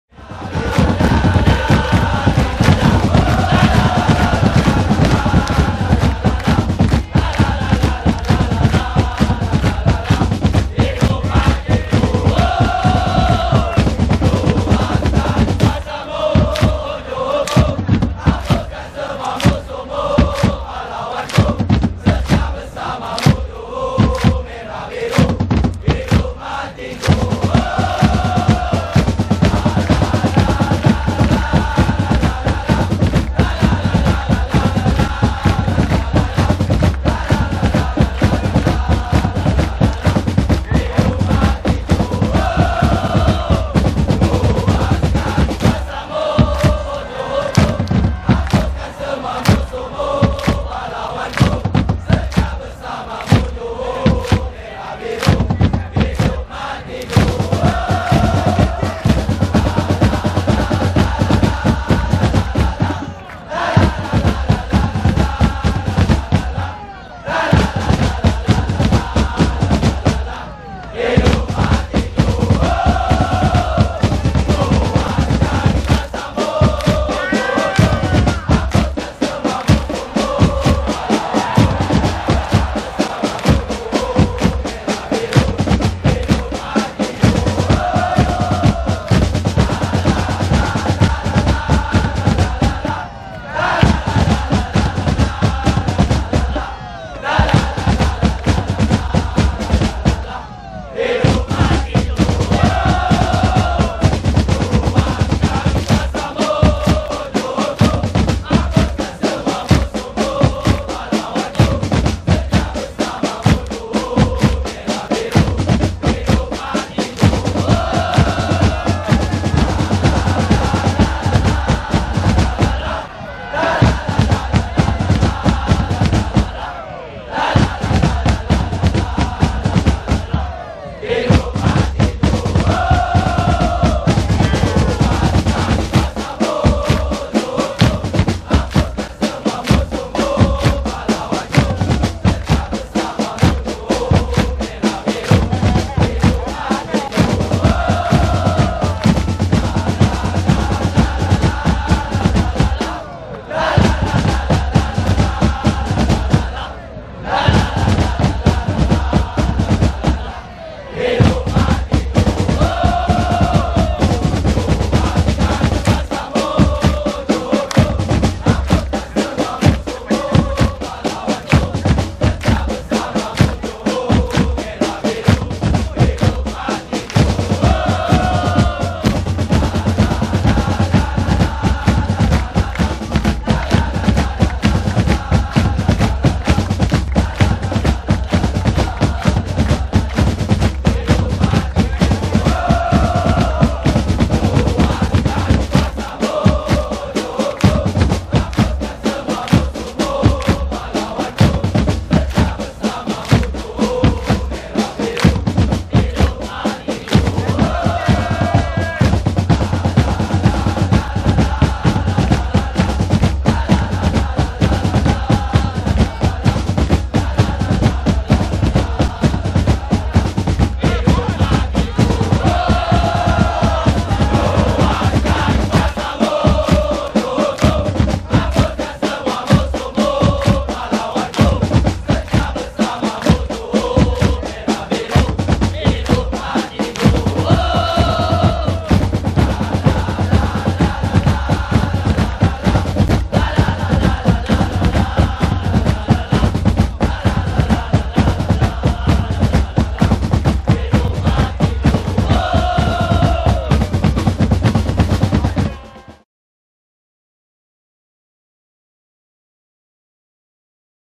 BOS FAMILIA Awaydays Melaka United vs JDT part Hawau 1
comrades Boys Of Straits mengegaukan away days Melaka United